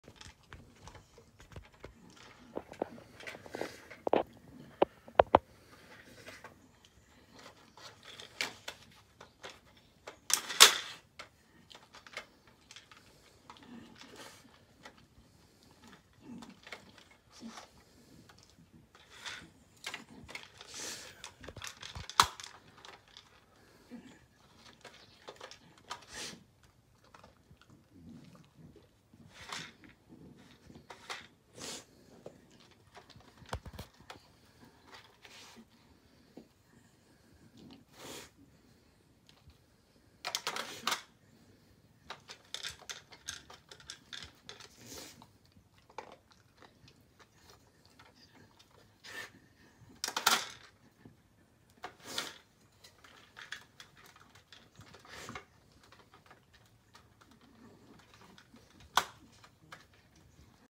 Puzzle treat time
Sometimes dad makes me use my brain to find da food.